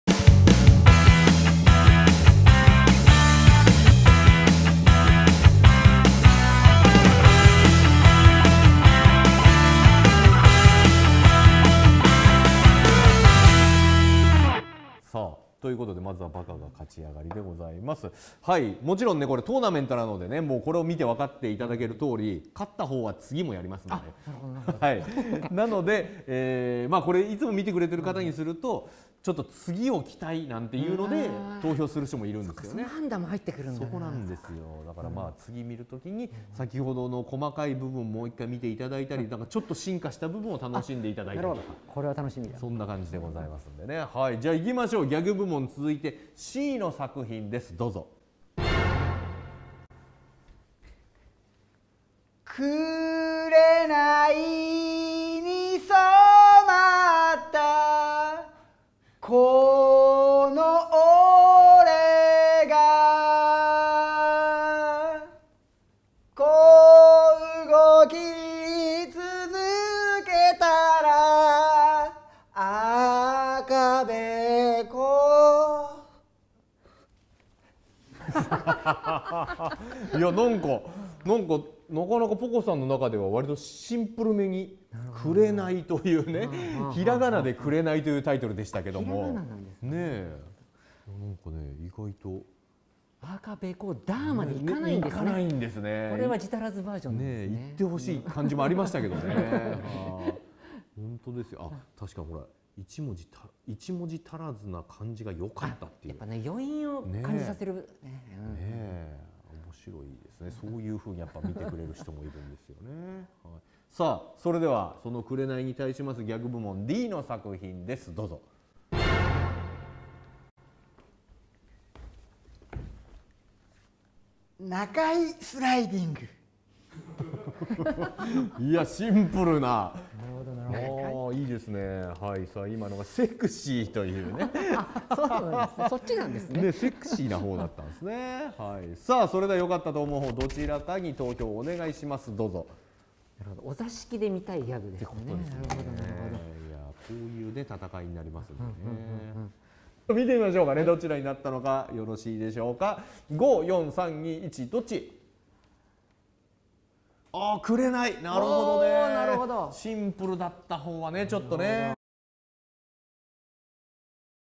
歩子1 2022-23 4月 ギャグ部門 1-2
スーパー天才芸人歩子の何が一番面白いのかを極めている史上最も頻繁に開催されているお笑いガチンコトーナメント歩子1gpが今シーズンも始まりました！